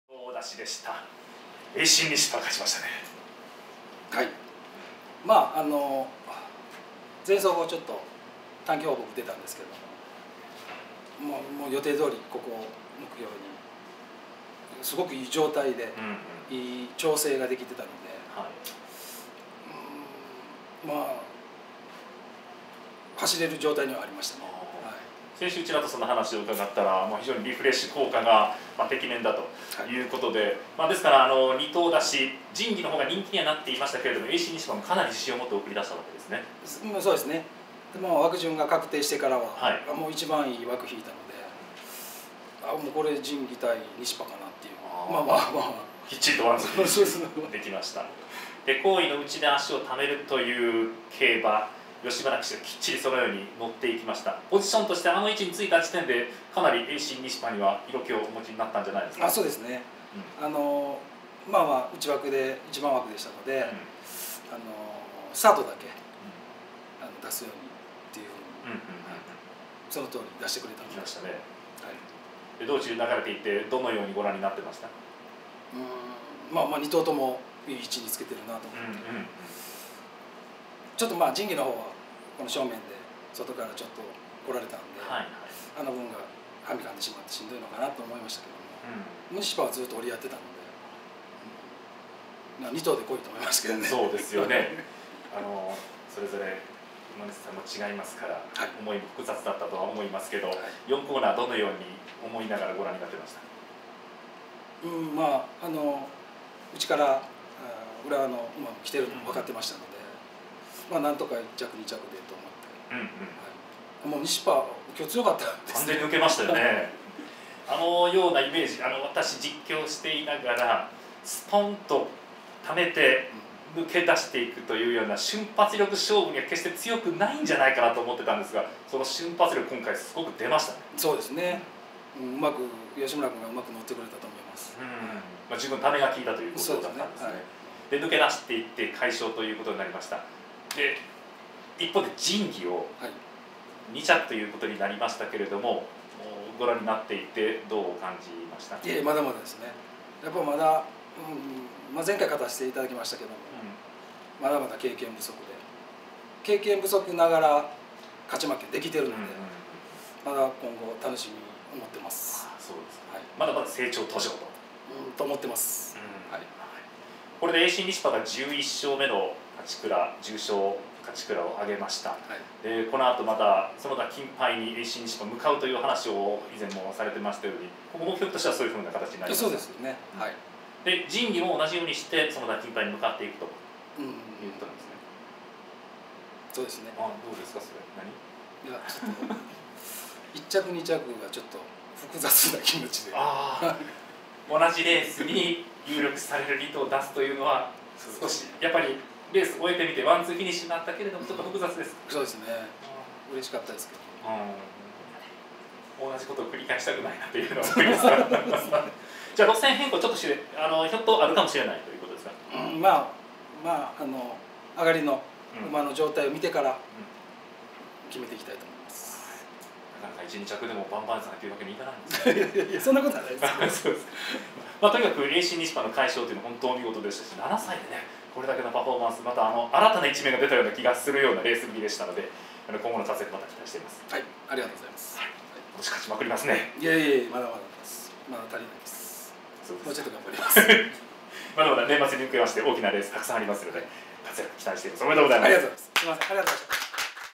第60回 姫山菊花賞 勝利調教師インタビュー
第60回 姫山菊花賞をエイシンニシパ号で制した橋本忠明調教師のインタビューです。